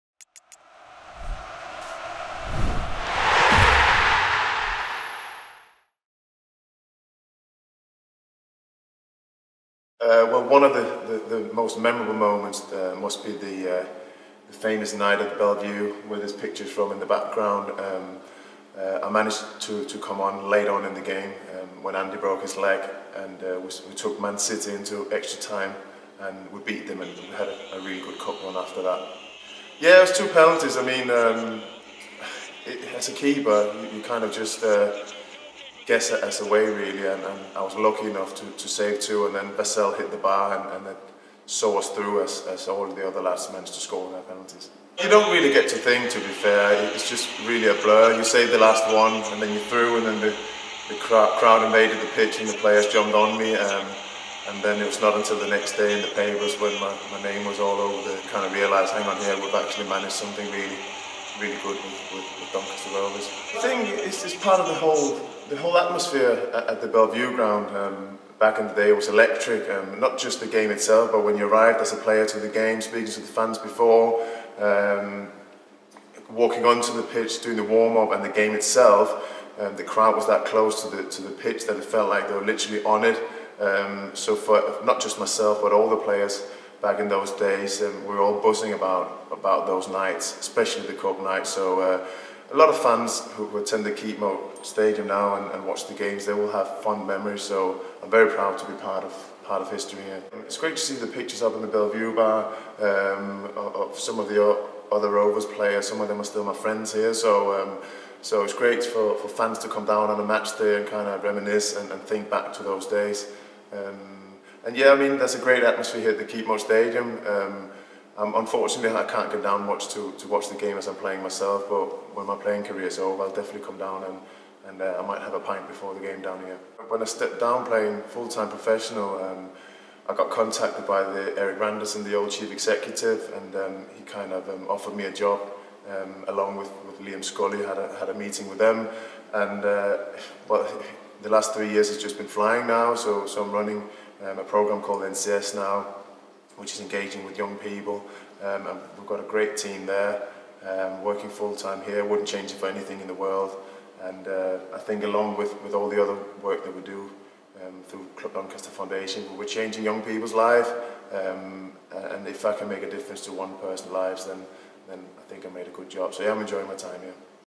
Jan Budtz on Belle Vue refurbishment
The Belle Vue Bar has been refurbished, with memorable moments from Rovers' history. Goalkeeper Jan Budtz features after a memorable cup game against Manchester City.